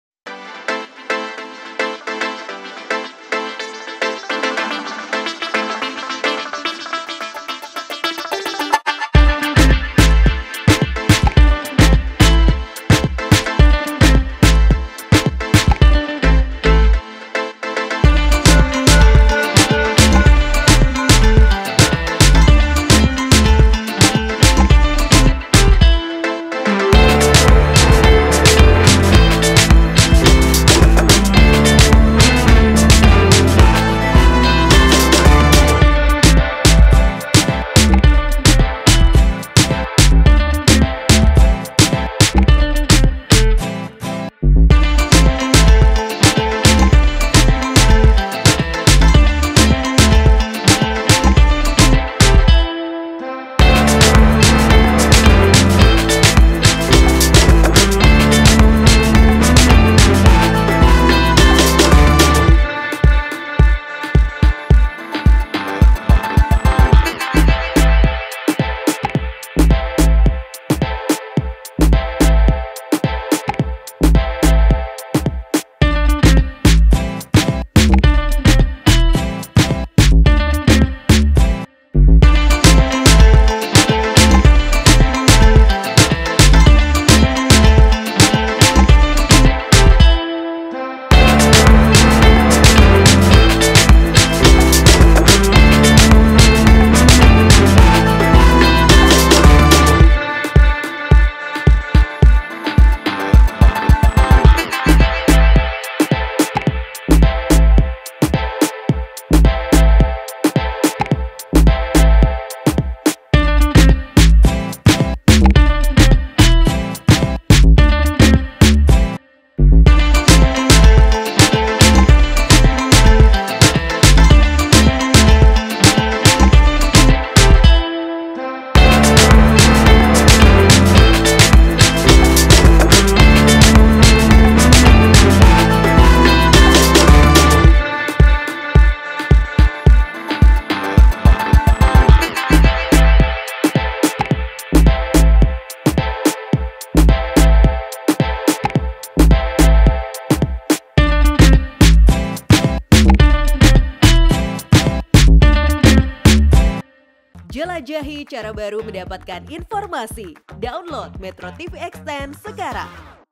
Cristiano Ronaldo Rayu Dua Eks Real Madrid Gabung Al Nassr
MetroTV, Meskipun menjalani musim individu yang fantastis, Al Nassr kembali gagal menjuarai Liga Pro Saudi. Tak puas dengan hasilnya, Cristiano Ronaldo mengambil tindakan sendiri untuk memperkuat timnya.